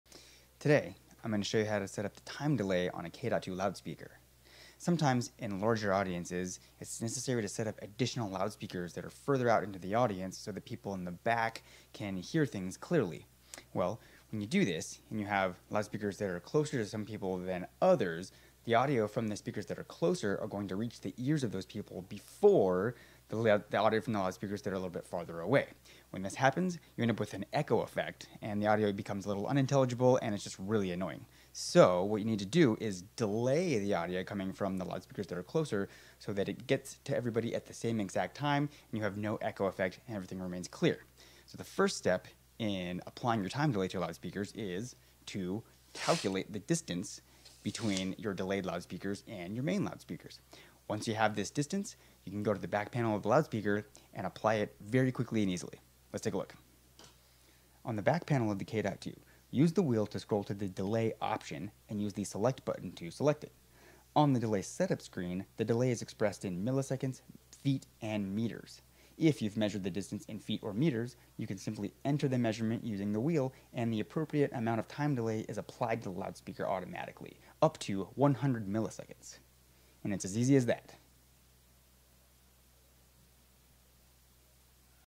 How To Set the Delay Time on K.2 Series Loudspeakers - Quick Tip
Learn how to set the delay time on K.2 Series loudspeakers (K8.2, K10.2,  K12.2), in order to time-align delay speakers or fill speakers to your main speakers at the stage.